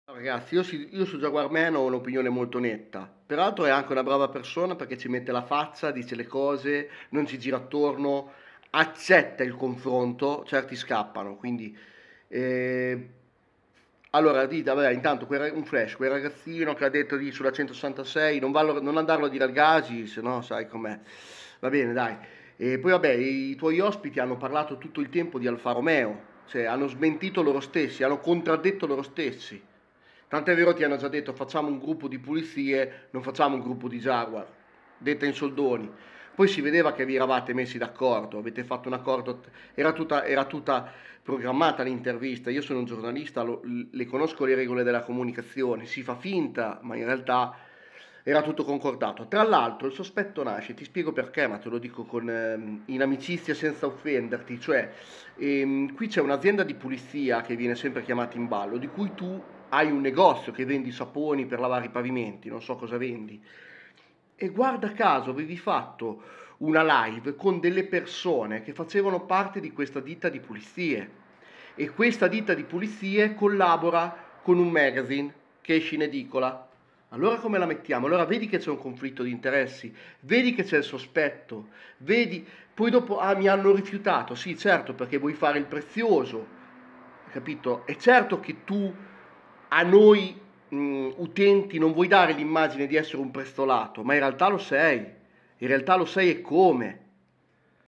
Jaguar Man | IL FANS CLUB È UNA CACATA PAZZESCA
Jaguar Man

La Lincoln era gestione Ford!!

Quindi Mondeo??

X100 migliore auto di cosa??

Altra Ford..!!

Al prox video!! Cambia squadra perché mi sembra abbastanza modesta!!Solo Alfa Romeo...Questi le Jaguar non sanno proprio cosa sono!!

Salutami GASI

Non sei fesso!! Se pigli sordi..Mica ce lo vieni a dire??

Che poi non ci sarebbe nulla di male... è solo una questione di credibilità...

Quanto all' x-Type...Guarda la Rover 75...gestione BMW...| FORD

Quindi che Jaguar è??

Guarda a caso tuti i gruppi creano FANS CLUB...Poteva essere più credibile se non vi conoscevate prima!! ahaha( gli ospiti erano presenti anche ad altri incontri guarda a caso..)